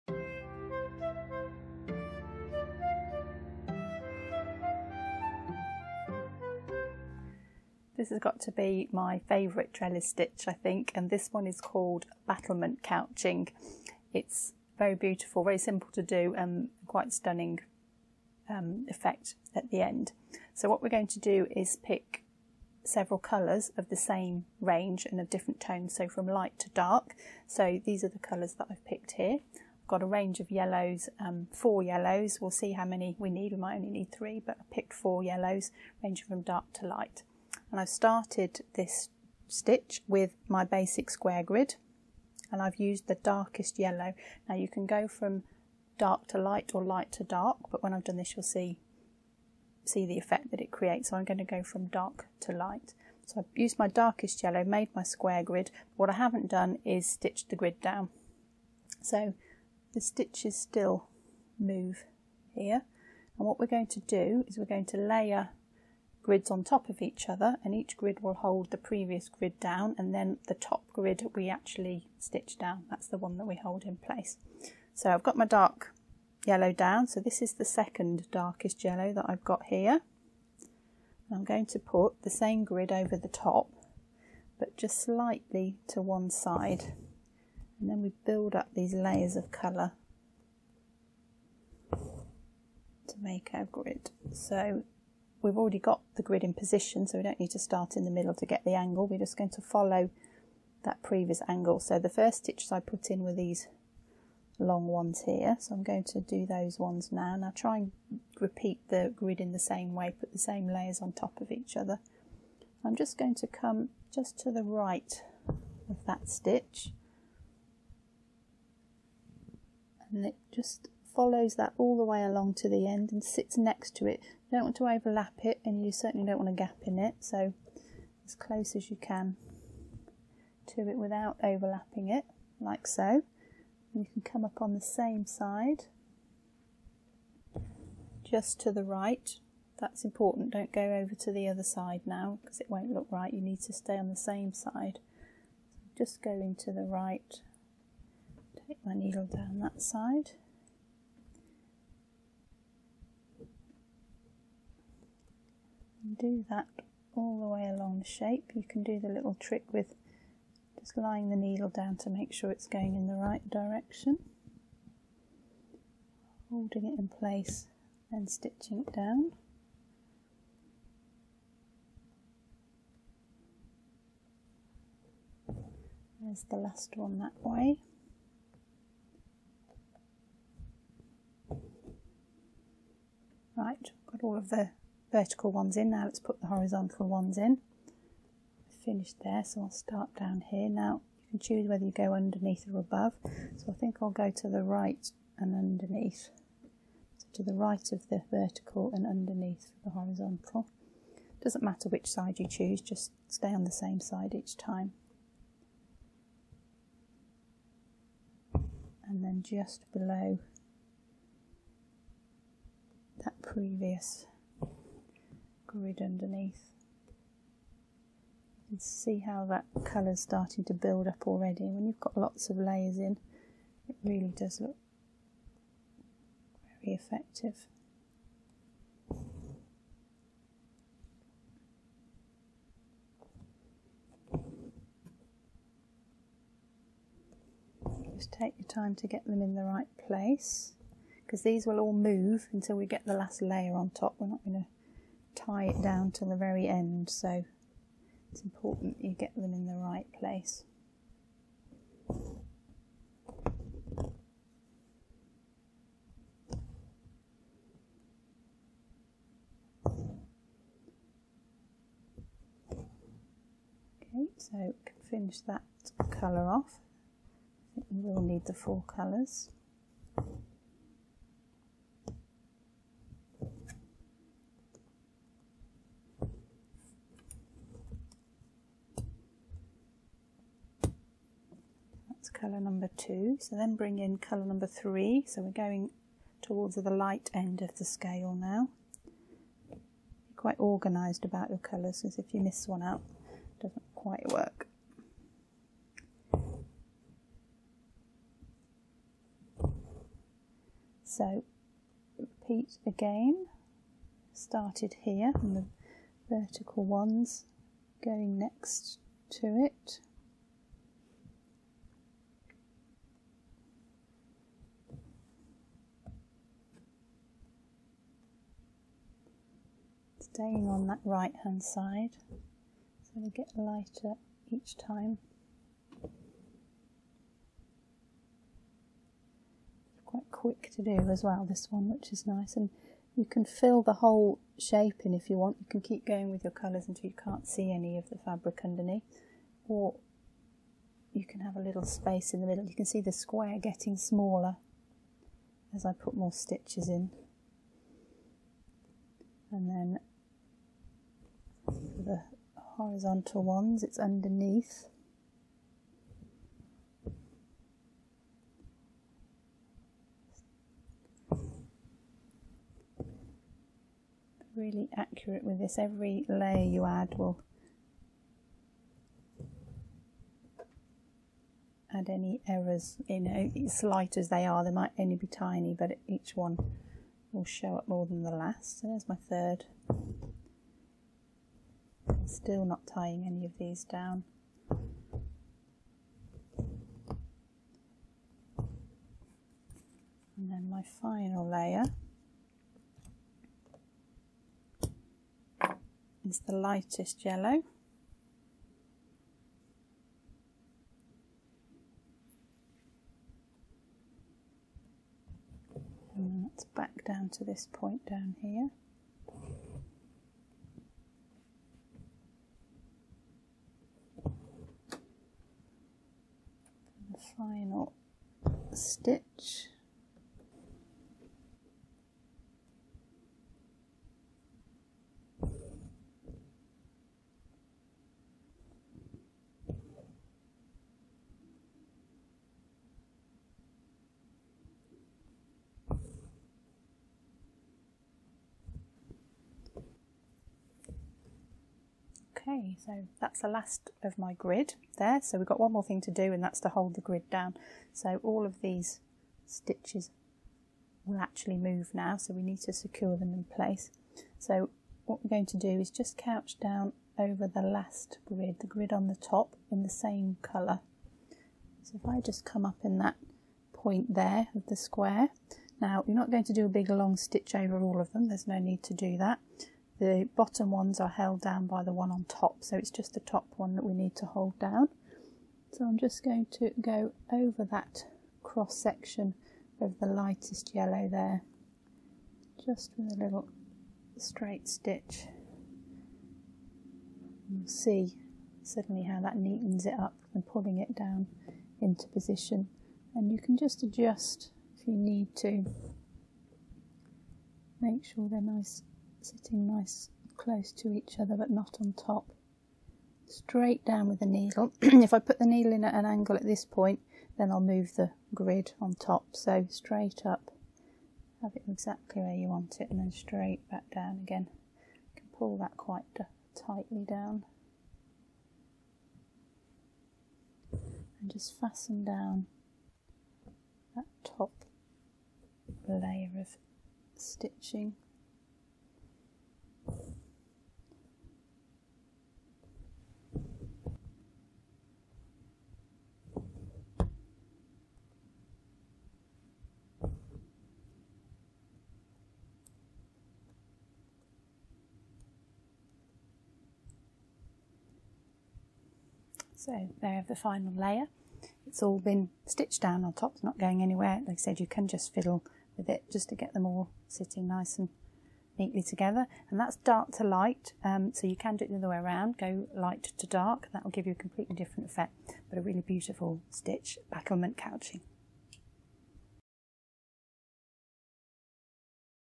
Hand Embroidery - Trellis Pattern #8 Battlement couching | Hand Embroidery Video Tutorial
How to hand embroider Battlement couching in Trellis stitch.

Presented by Sarah Homfray, a graduate of The Royal School of Needlework 3-year apprenticeship and a professional hand embroiderer and  tutor.

for your hand embroidery supplies.
Hand embroidery for beginners and experts!